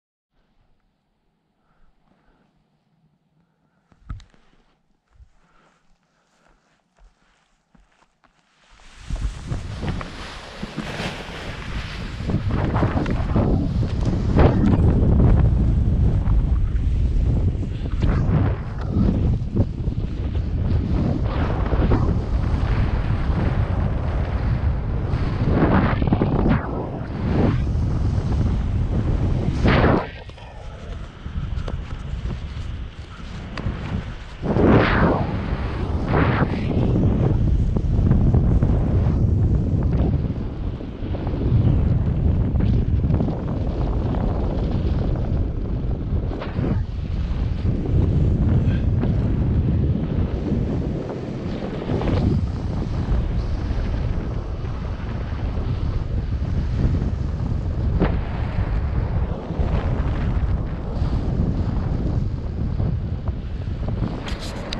Pointe Blanche : déco montagne et court !
Superbe sommet, conditions de vent moyennes (travers faible), pas de face voile, dos voile mais course d'élan très courte !